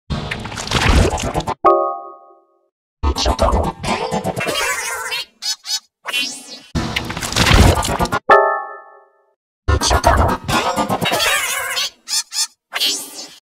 google hangouts csupo effects
i use google hangouts csupo because i used hangouts for trve and voitejch to choose the effects

here is the list:
1. new mason major
2. sonic major